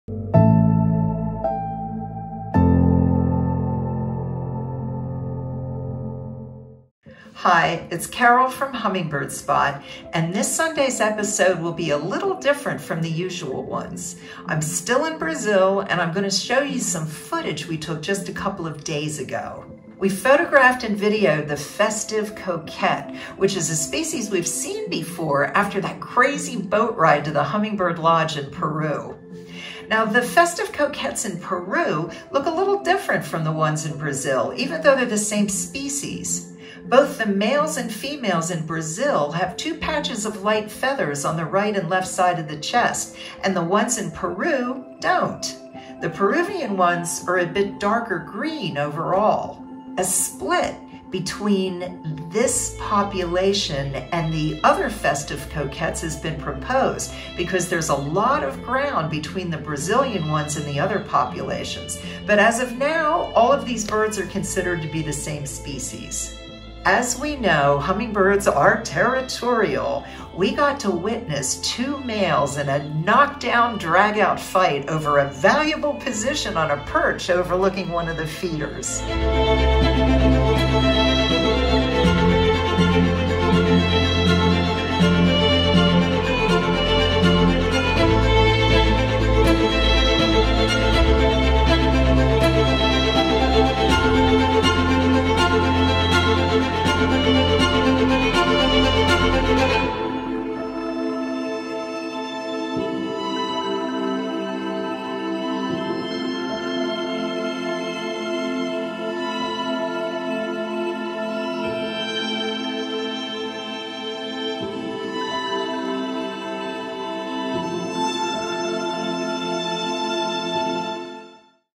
Brazil's Festive Coquette Featuring Rare Footage of a Hummingbird Fight
We are currently traveling in Brazil to find beautiful hummingbirds to photograph.  A few days ago we again encountered the Festive Coquette and got incredible footage of a fight between two males.